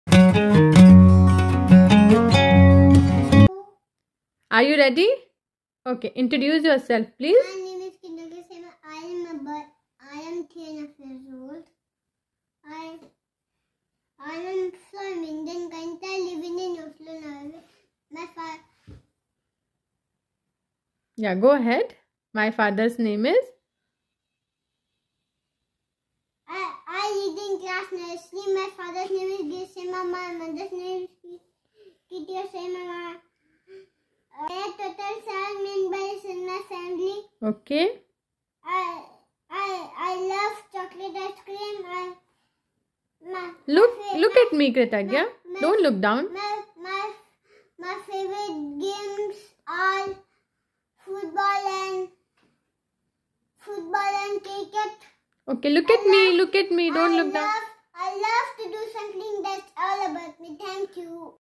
Short Video To Teach Self Introduction To Pre-school and Nursery Kids
Hello Everyone,

In today's video I will show how to teach Self Introduction to pre-school and Nursery Kids.